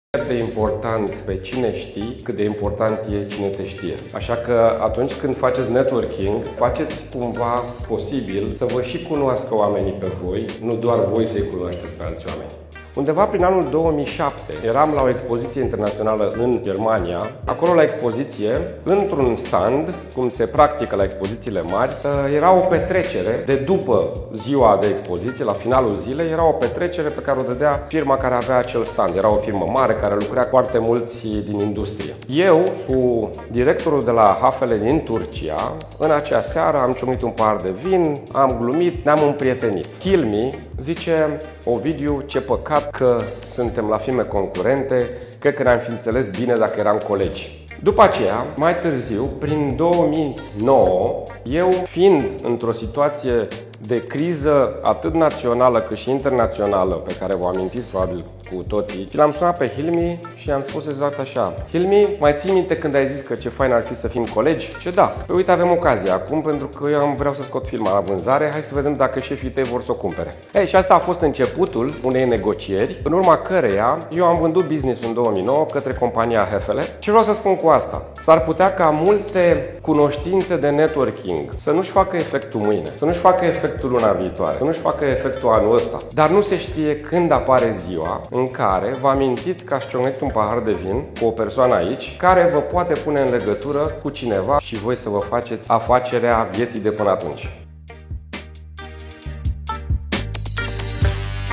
👉🏻 Networking-ul eficient nu înseamnă doar să cunoști pe alții!
👉🏻 Networking-ul eficient nu înseamnă doar să cunoști pe alții, ci și să te asiguri că și ei te cunosc pe tine, să existe un schimb bilateral de valoare și cunoaștere.